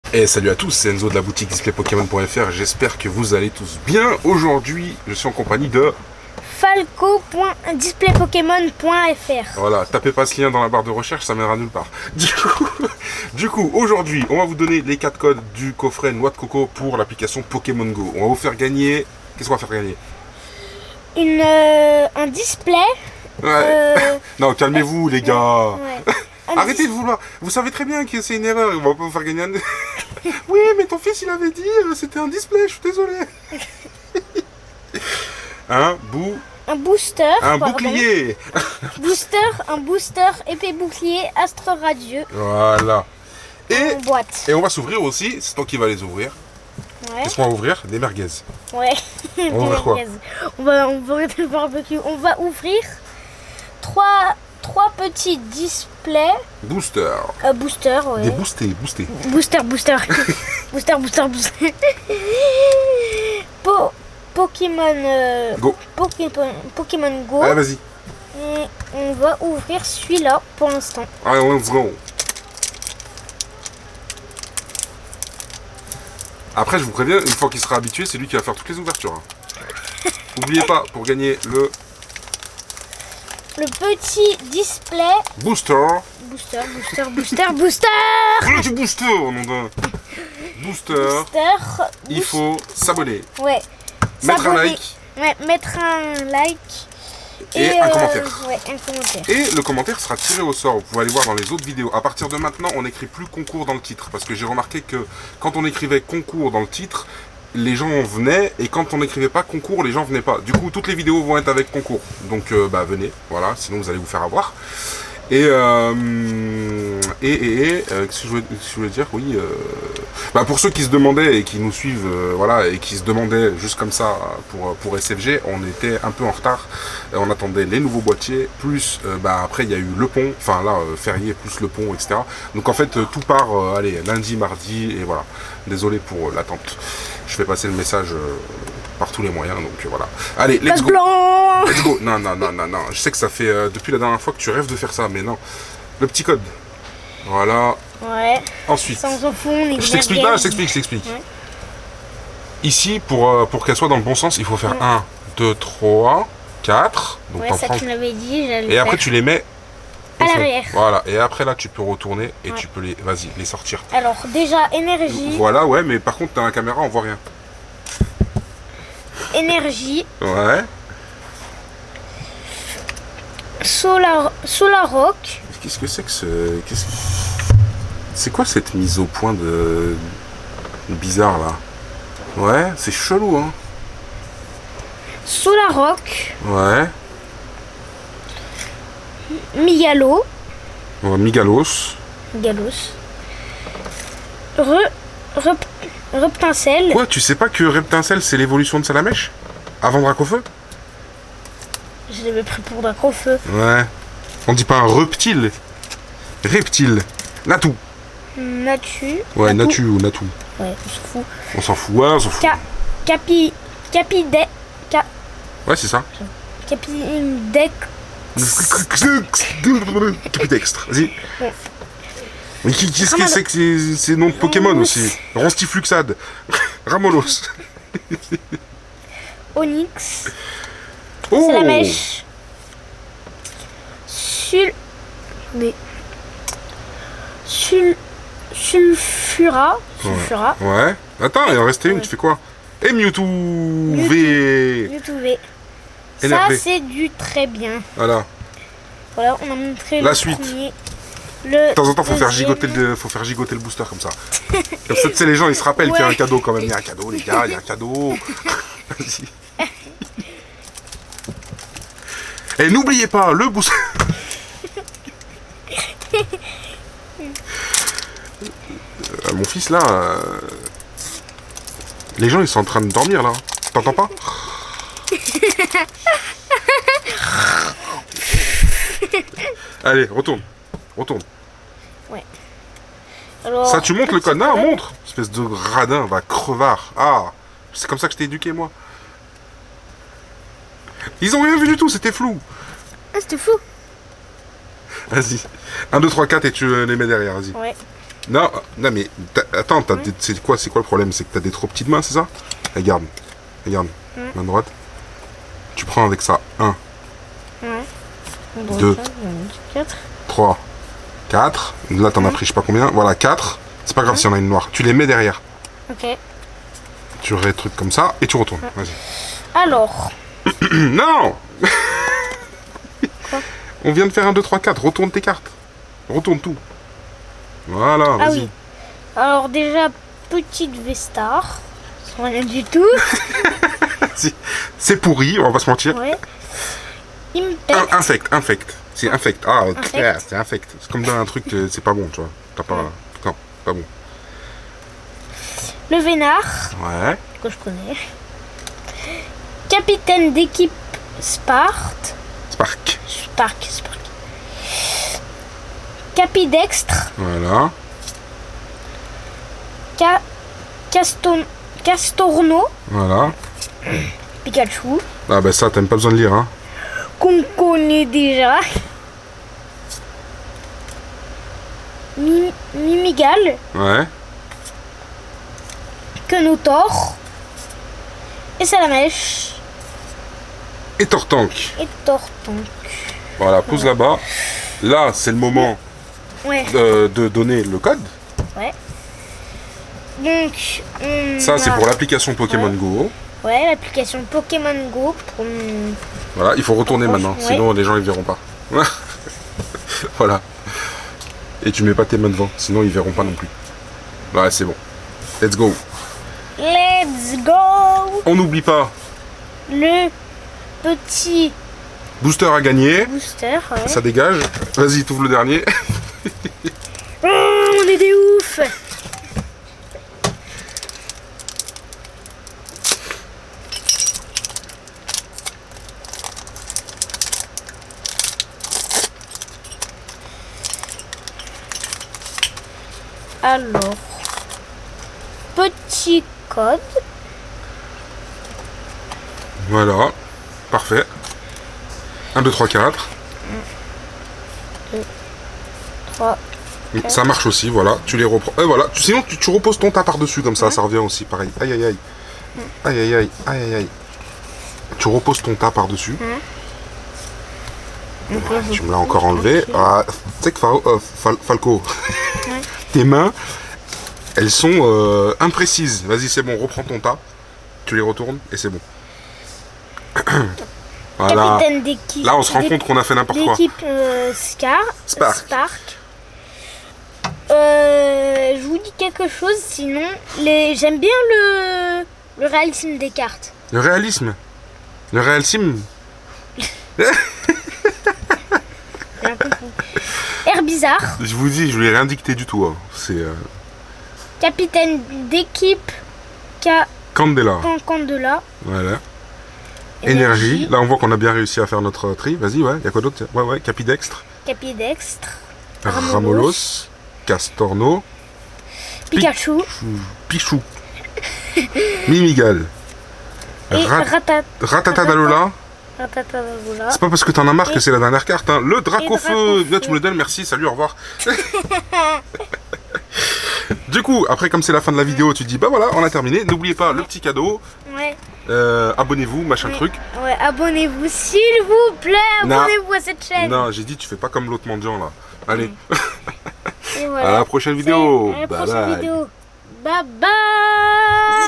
Ouverture Cartes Pokémon GO lessssgoooO
On ouvre des booster pokémon
On ouvre des coffrets pokémon ETB
On ouvre des display pokémon
On présente des produits
On s'amuse